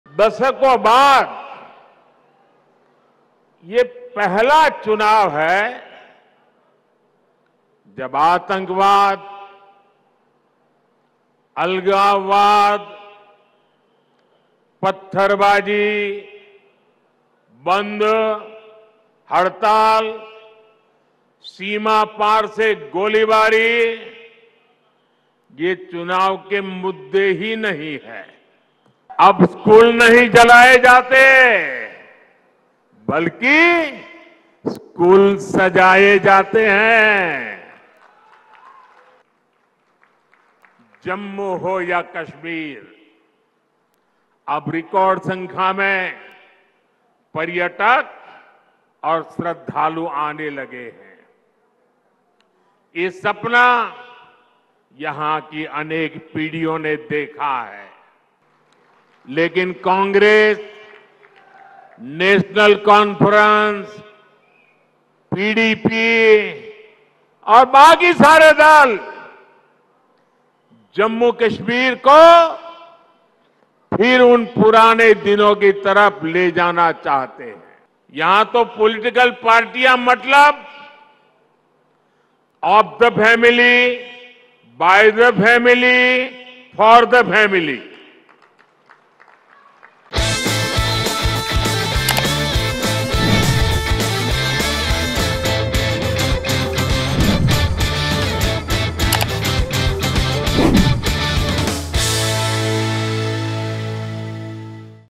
Prime Minister Narendra Modi's Call For 'Naya Kashmir', Slams Opposition
Prime Minister Modi champions the vision of 'Naya Kashmir', emphasizing the shift away from stone-pelting and cross-border terrorism as election issues. He criticizes parties led by Mehbooba Mufti and Omar Abdullah for engaging in divisive politics, signaling a departure from the past and a commitment to a new era of development and progress in the region.